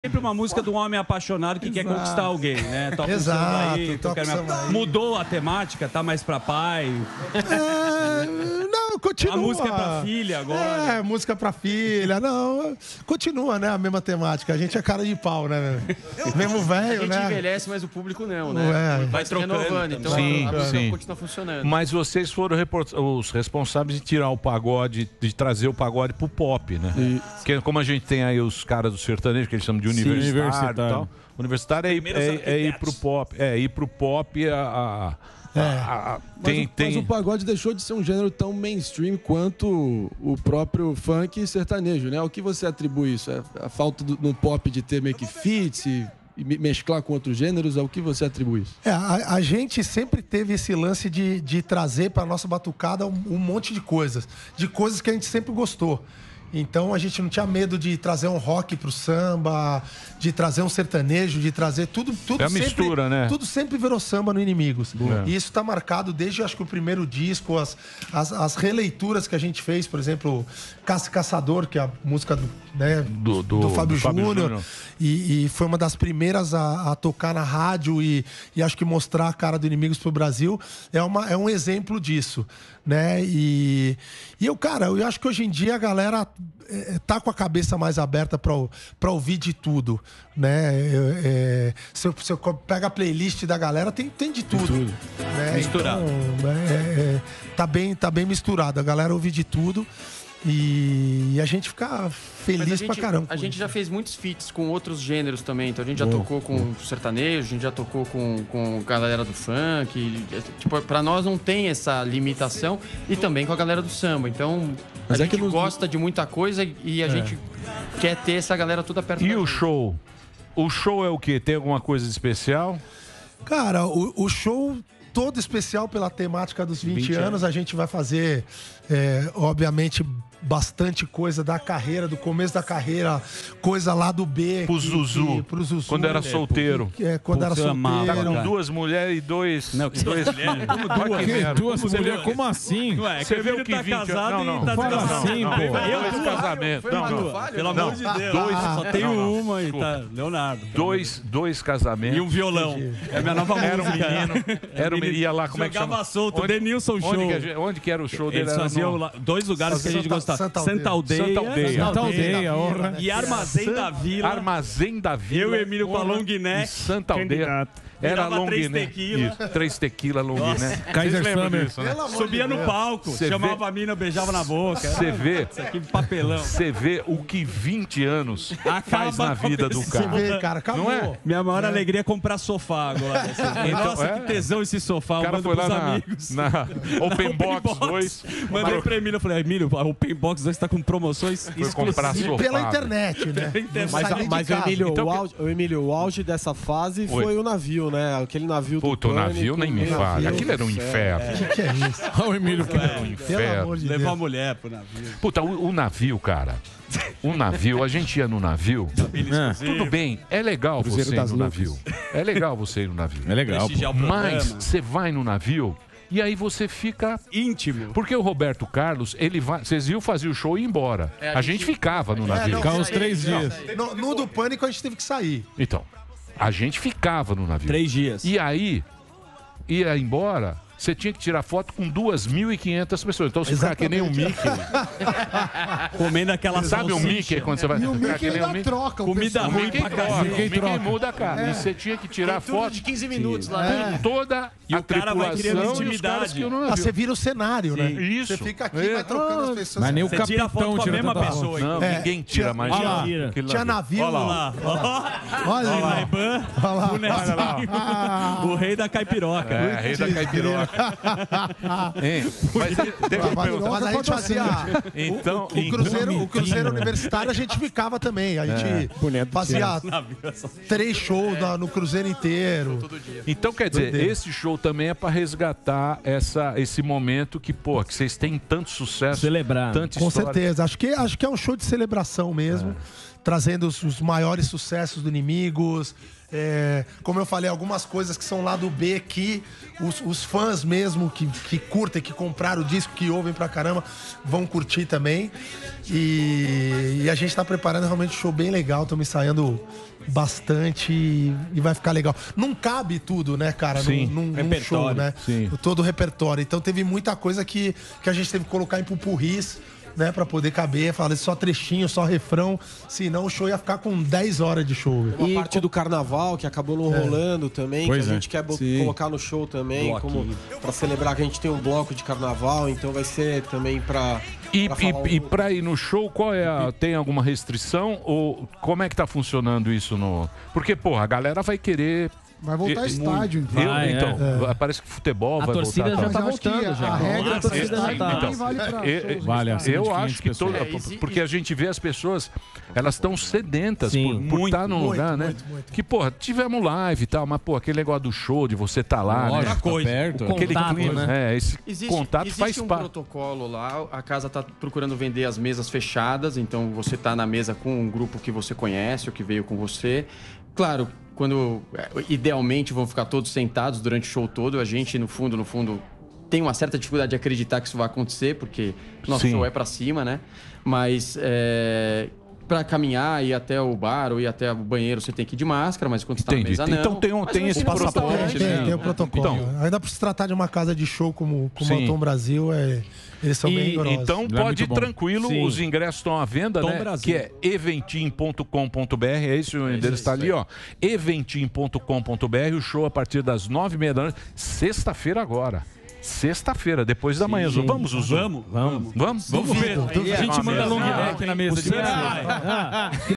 20 ANOS EM UMA NOITE: Inimigos da HP falam sobre SHOW ESPECIAL DA CARREIRA
Os integrantes do Inimigos da HP falam sobre o diálogo que sempre tentaram promover entre o samba e outros gêneros musicais, e falam sobre o que está por vir no show especial de 20 anos de carreira, que acontecerá nesta sexta-feira (1º), ás 21h30 no Tom Brasil, em São Paulo